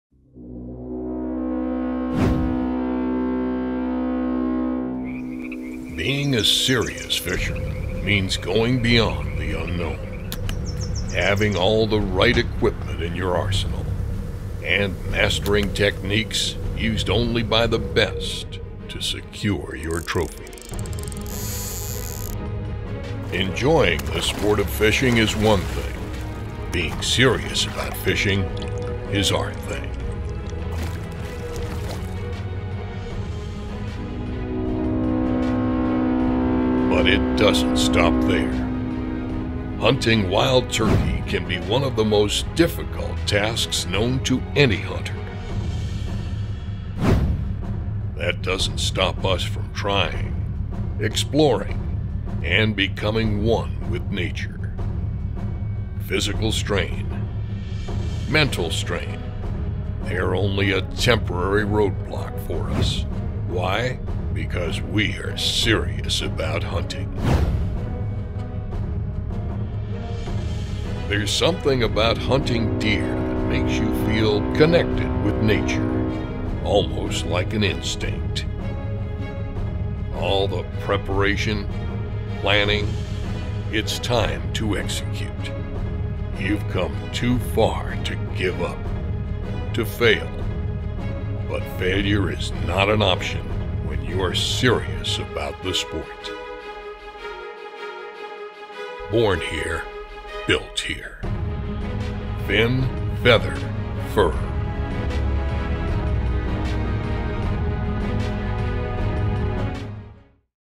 Fin Feather Fur Commercial
An Ohio based company that sells products for Hunting, Fishing, Camping etc.